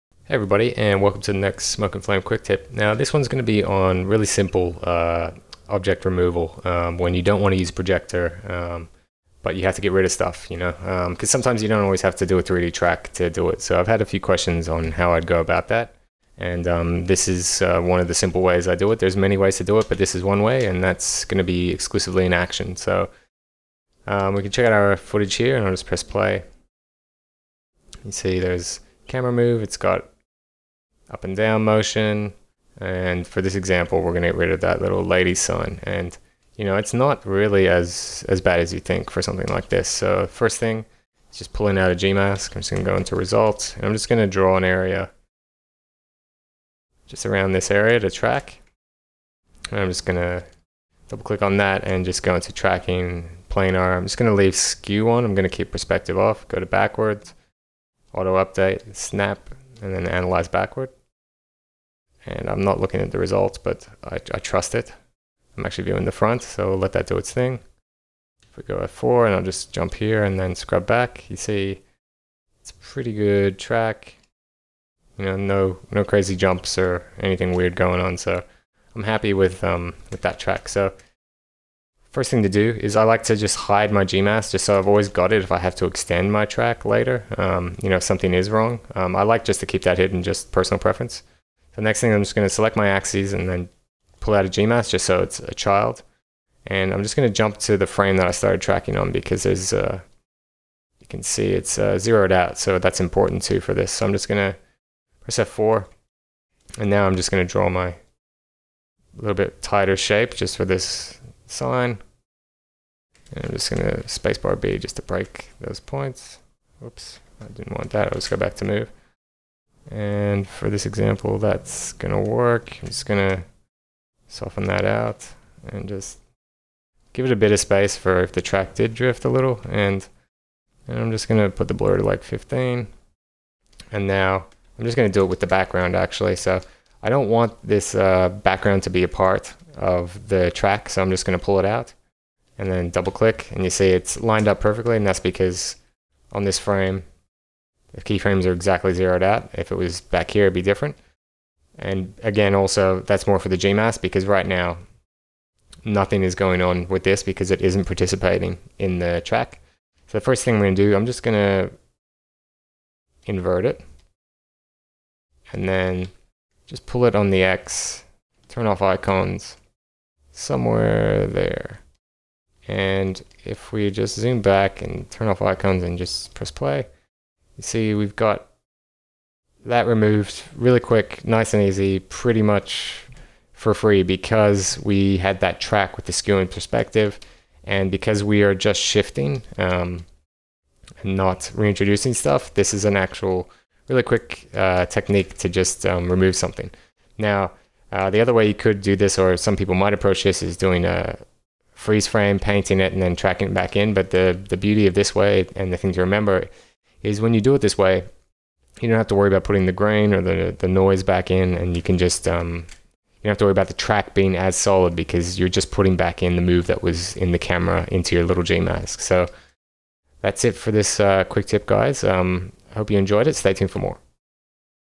simple object removal in action
a quick look at one of many ways to perform object removal inside of action